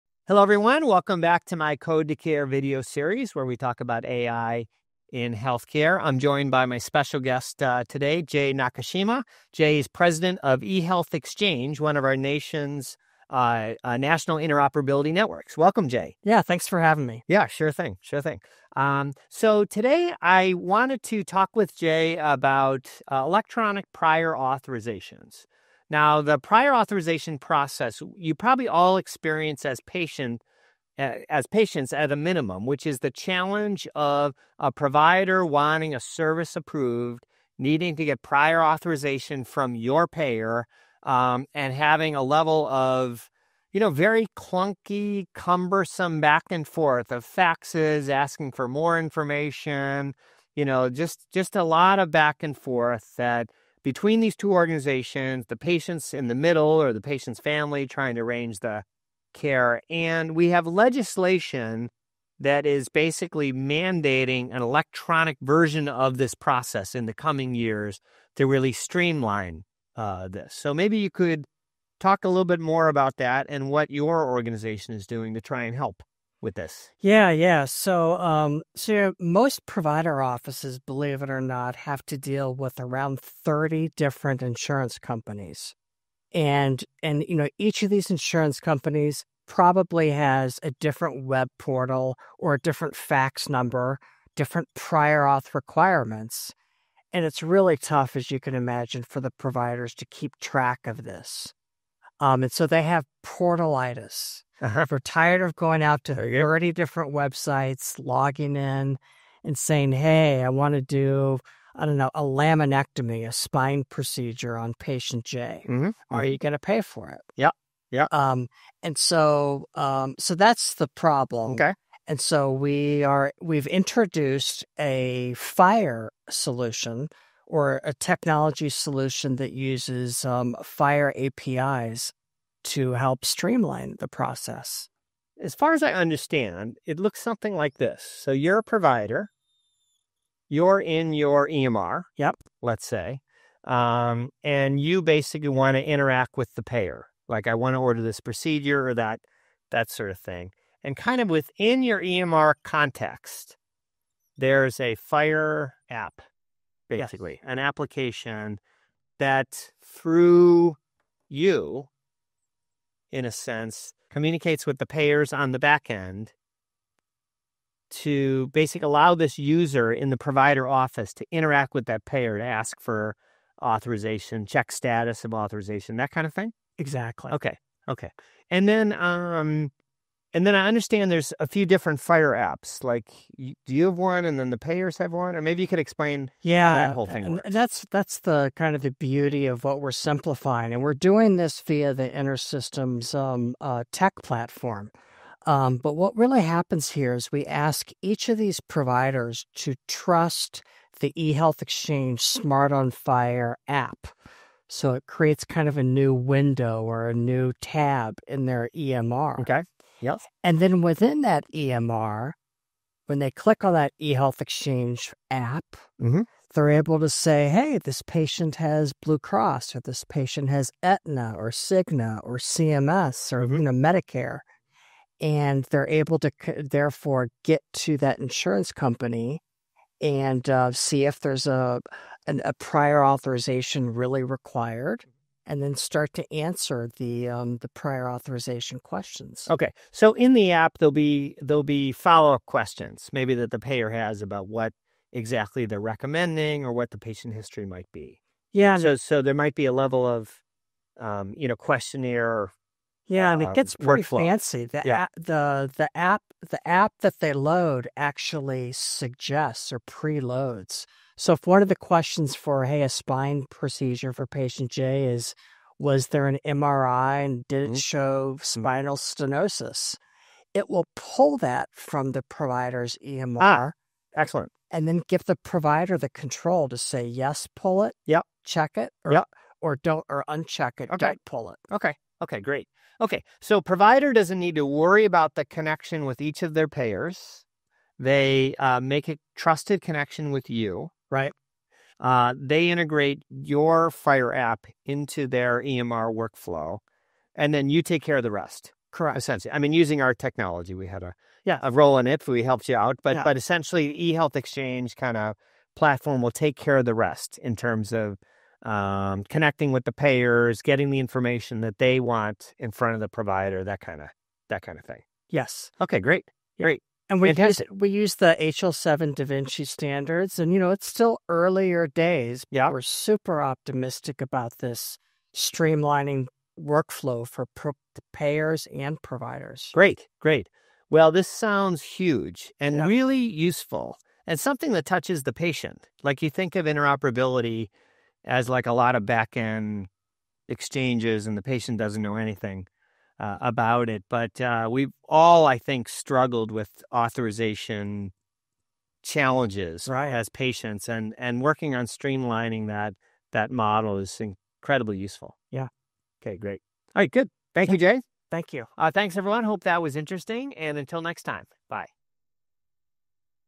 Streamlining Electronic Prior Authorizations with FHIR
Explore how FHIR-based technology is transforming the cumbersome prior authorization process in healthcare. Learn how their innovative SMART on FHIR app streamlines payer-provider communication, reduces administrative burden, and ultimately improves the patient experience. Discover how the collaboration leverages InterSystems technology to simplify and standardize this critical interaction, helping providers navigate complex payer requirements in real-time - all from within their EHR.

Presenters:
🗣  Don Woodlock, Head of Global Healthcare Solutions, InterSystems
🗣  Jay Nakashima, President, eHealth Exchange, Board Member